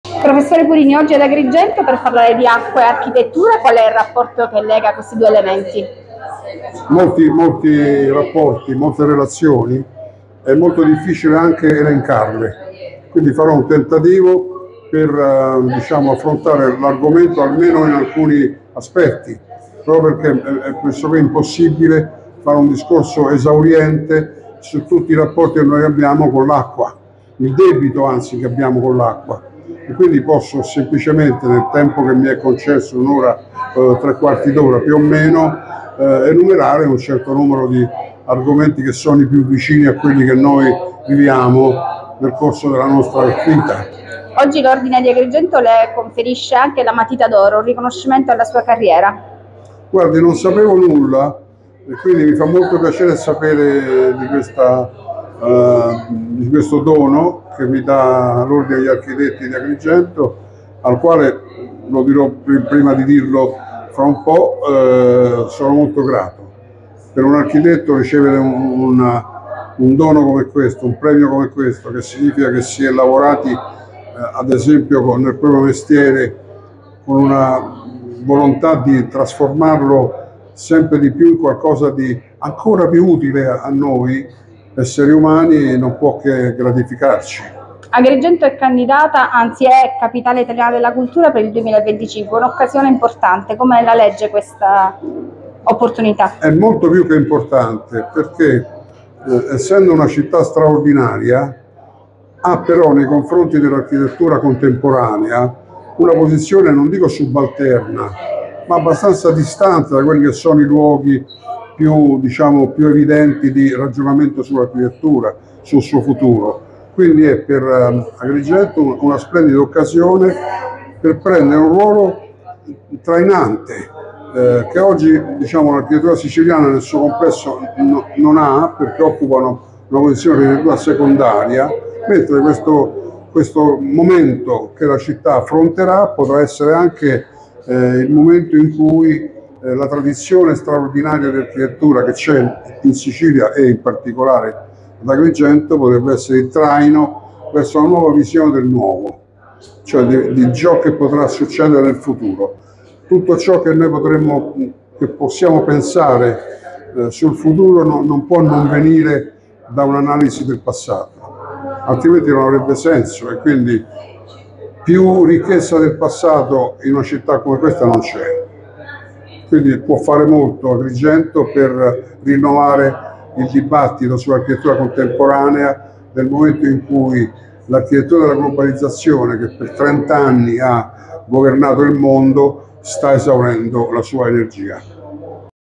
“Acqua e Architettura” Lectio Magistralis del Prof. Franco Purini ad Agrigento_in3minuti.it
Lectio Magistralis, sul tema del rapporto tra “Acqua e Architettura”, tenuta dal Prof. Franco Purini, uno dei più celebri esponenti  del neorazionalismo italiano. Ascoltiamolo al microfono di Giovanna Neri in3.minuti.it